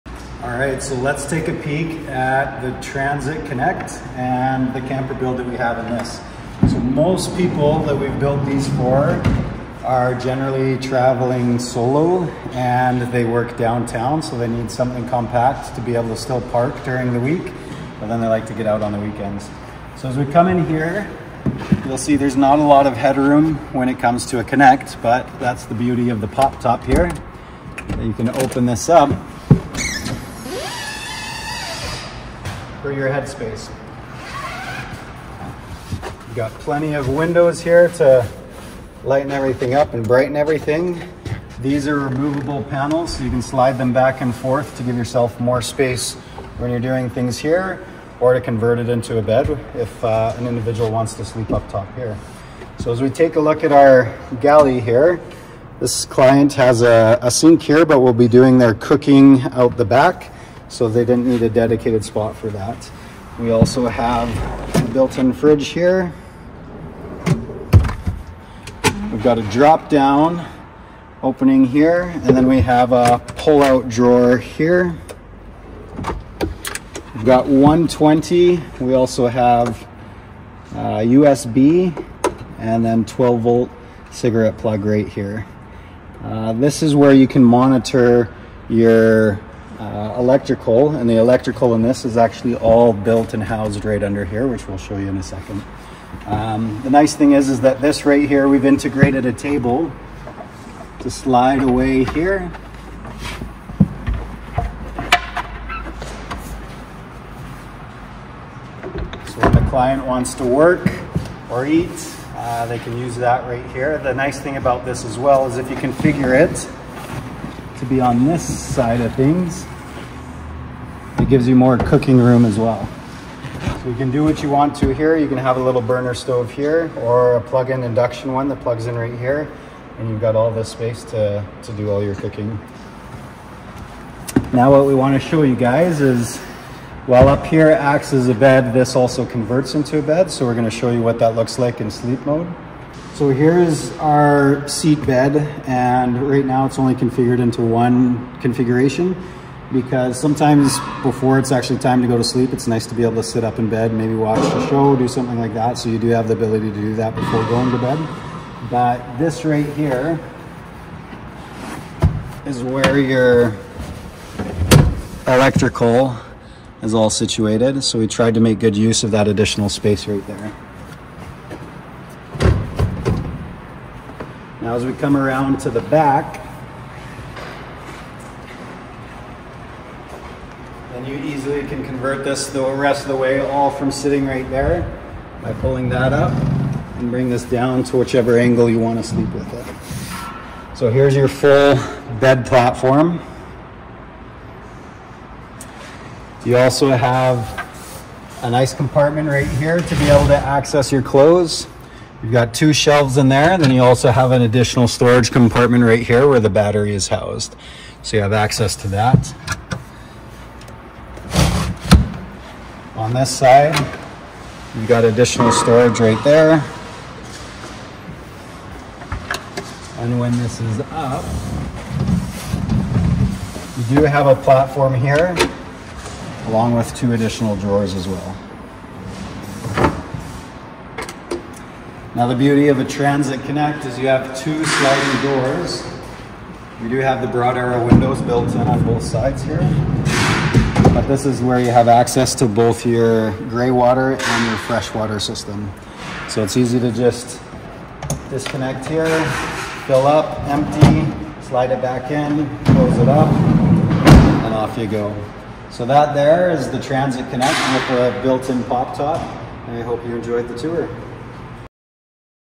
Transit Connect with a Pop Top
A 2019 Transit Connect Cargo Van (Long Wheel Base) camper build. This particular build features a pop top and a West Coast Mini floor plan.